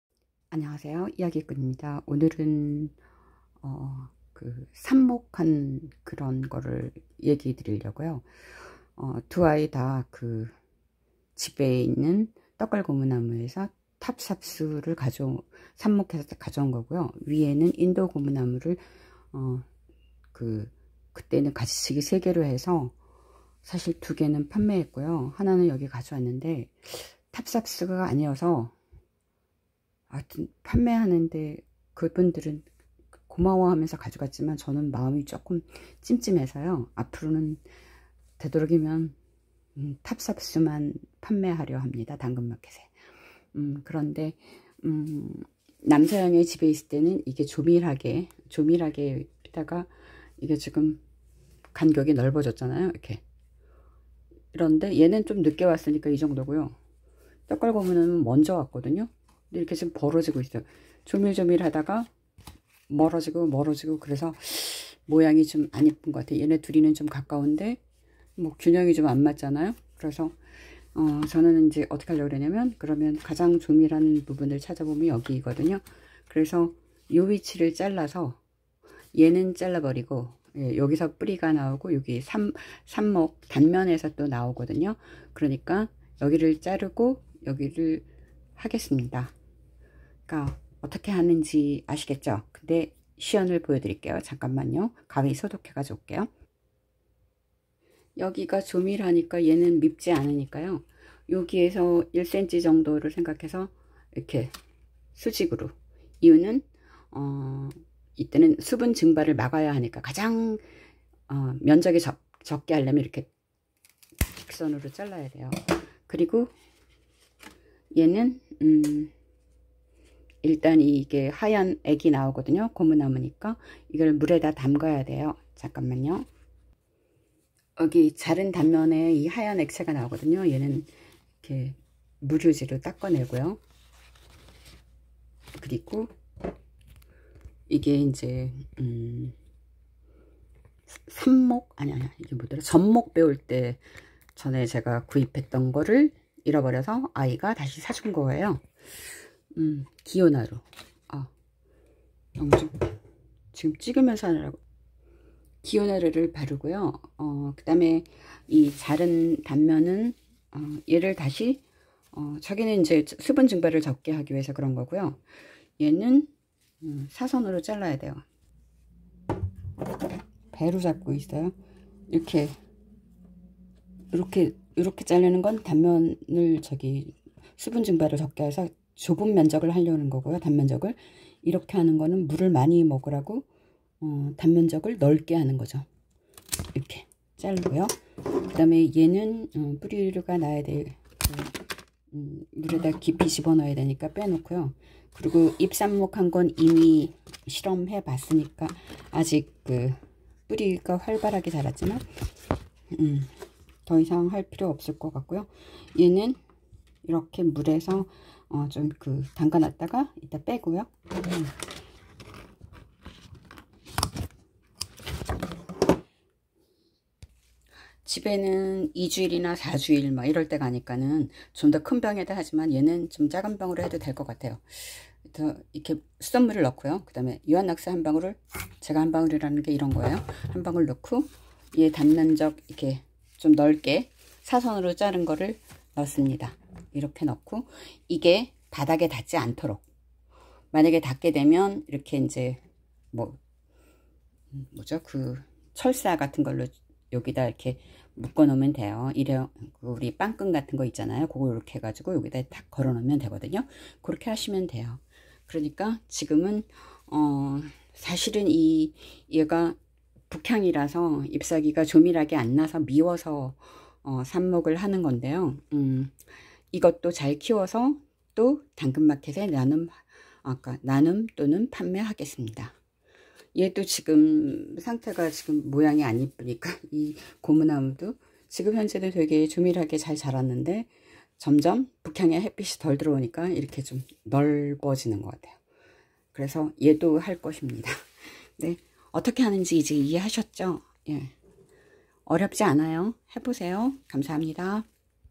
떡갈고무나무의 가지치기후 물삽목하기
북향이라서 햇빛이 거의 없어서 잎사귀간격이 벌어지므로 가지치기해서 물삽목을 해 본다.